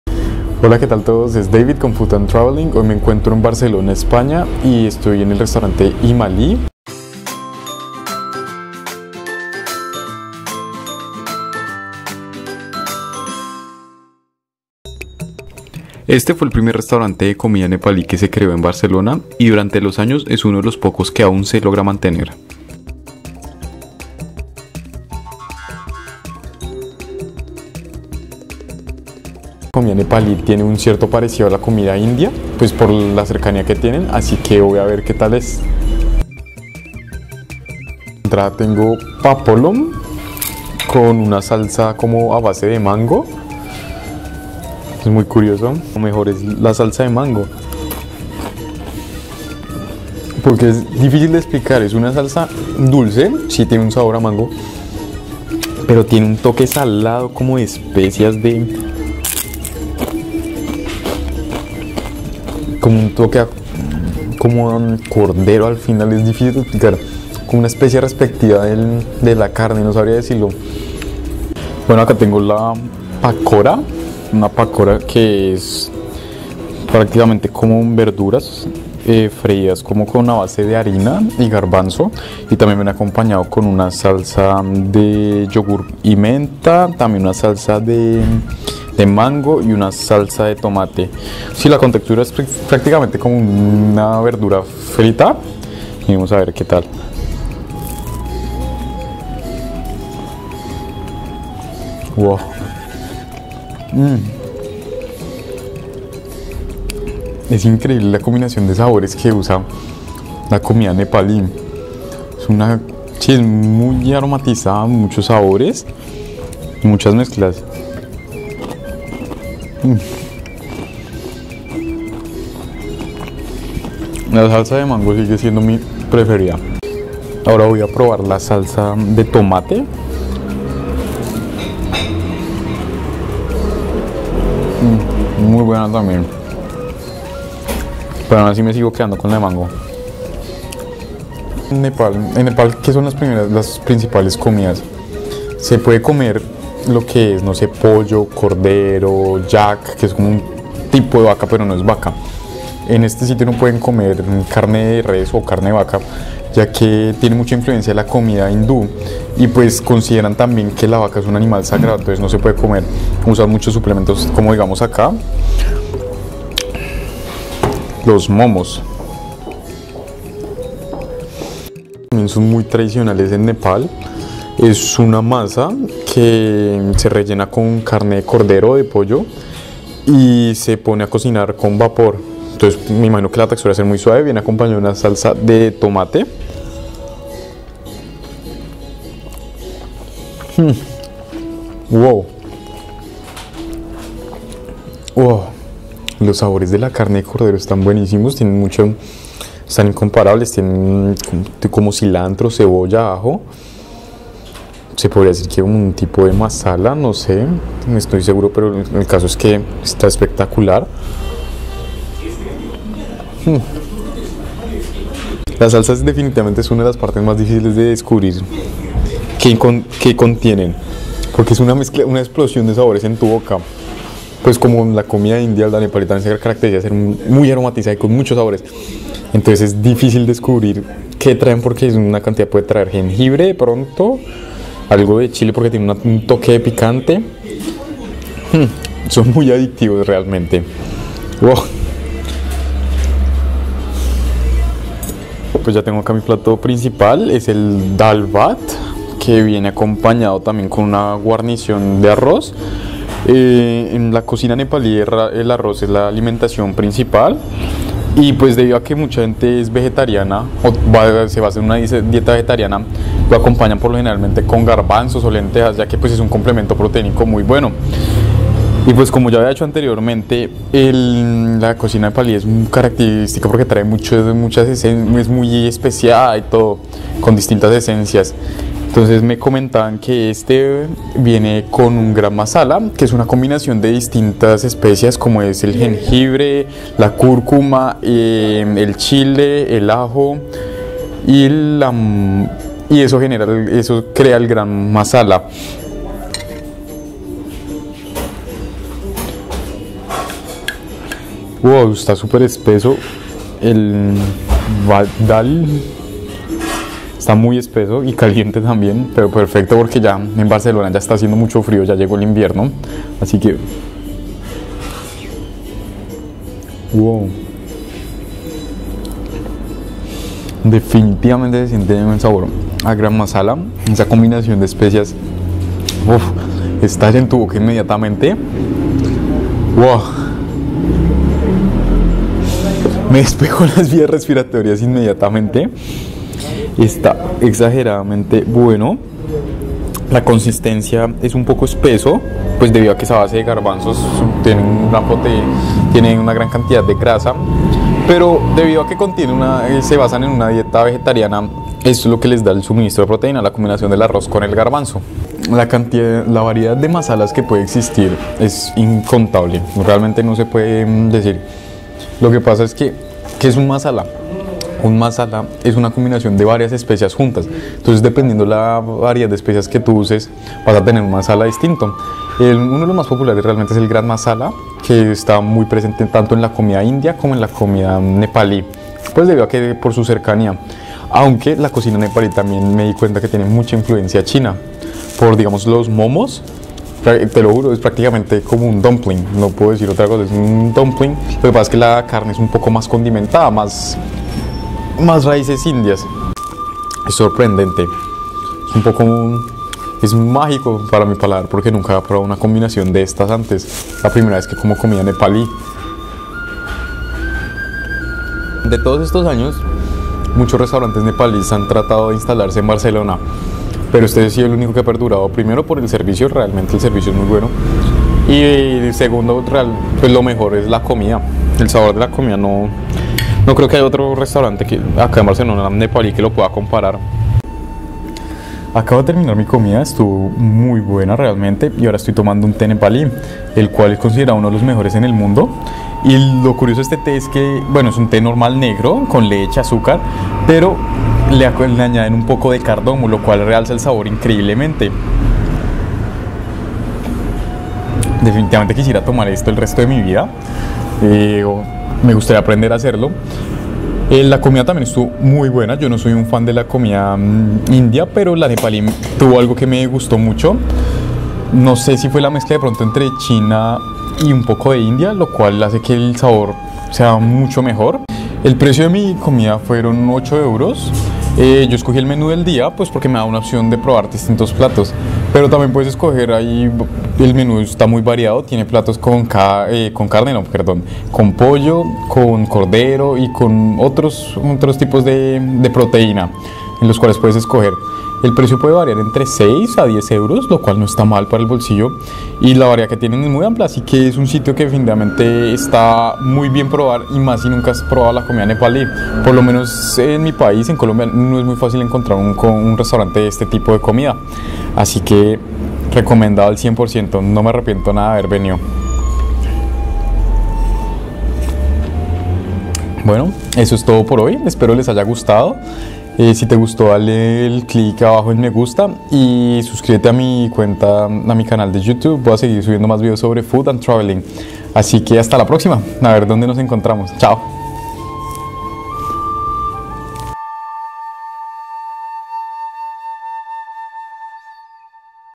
Dal Bhat - Irresistible Comida Nepali en Barcelona (English Subtitles)
HIMALI fue el primer restaurante  de comida Nepali que existió en Barcelona y es uno de los pocos que aun se mantiene en pie y esto se debe al la gran variedad y calidad que posee este restaurante.

Audio cortesia de Audio Networks